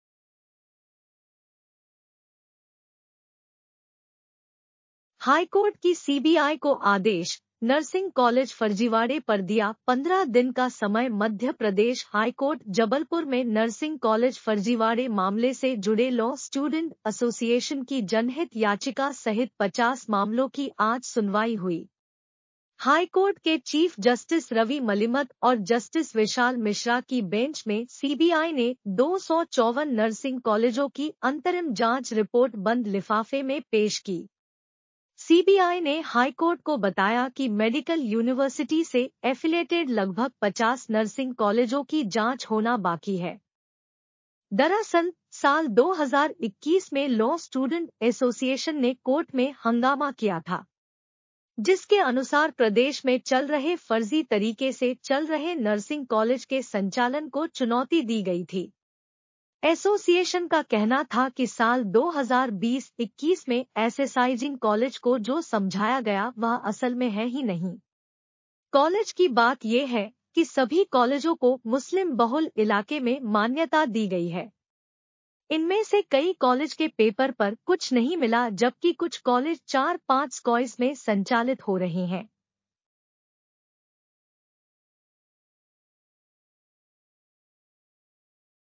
हाईकोर्ट की CBI को आदेश, नर्सिंग कॉलेज फर्जीवाड़े पर दिया 15 दिन का समय
मध्य प्रदेश हाईकोर्ट ने गुरुवार को सीबीआई को नर्सिंग कॉलेज फर्जीवाड़े की जांच 15 दिन में पूरी करने के आदेश दिए। इस मामले में लॉ स्टूडेंट एसोसिएशन की जनहित याचिका पर सुनवाई करते हुए कोर्ट ने यह आदेश दिया।

सीबीआई ने कोर्ट को बताया कि मध्य प्रदेश आयुर्विज्ञान विश्वविद्यालय से संबद्ध लगभग 50 नर्सिंग कॉलेजों की जांच होना बाकी है। इसके अलावा अन्य 50 कॉलेजों की जांच पर सुप्रीम कोर्ट की रोक लगी है।

कोर्ट ने सुनवाई के दौरान सीबीआई से कहा कि बच्चों के भविष्य को देखते हुए जल्द से जल्द जांच पूरी करनी चाहिए। कोर्ट ने सीबीआई को 15 दिन की मोहलत दी, लेकिन अगली सुनवाई के लिए दो सप्ताह का समय तय किया है।

इस मामले में हाईकोर्ट ने 2021 में एक आदेश जारी कर सीबीआई को जांच करने का निर्देश दिया था। सीबीआई ने अब तक 254 कॉलेजों की जांच पूरी कर ली है।

नर्सिंग कॉलेज फर्जीवाड़े मामले में यह आरोप है कि कई कॉलेजों ने बिना मान्यता के नर्सिंग की पढ़ाई शुरू कर दी थी। इन कॉलेजों में फर्जी तरीके से छात्रों को एडमिशन दिया गया था।